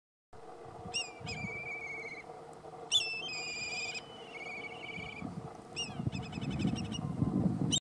Black Kite calling.
This Black Kite (Milvus migrans) was calling while standing on the power lines near the Long Trough and Prison Boab Tree, Derby, WA.